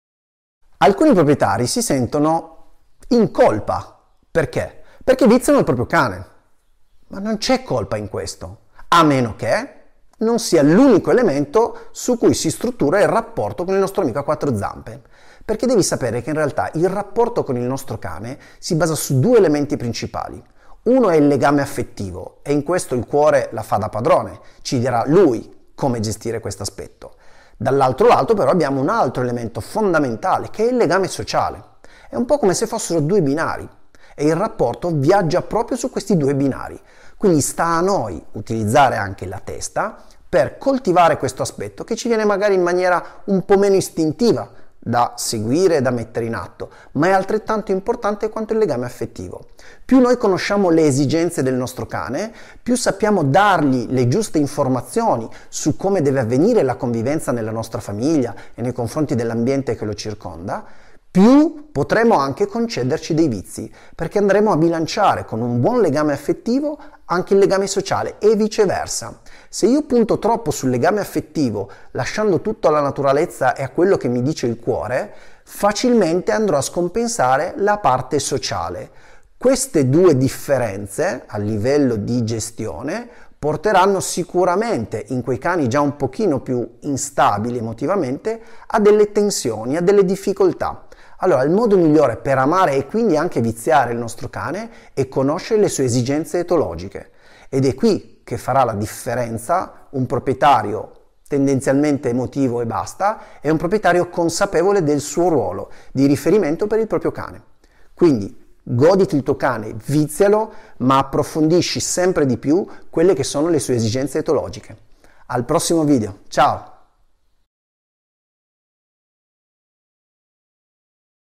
Posso viziare il mio cane?
Questa domanda è una tra le più gettonate tra i miei clienti, e la risposta è...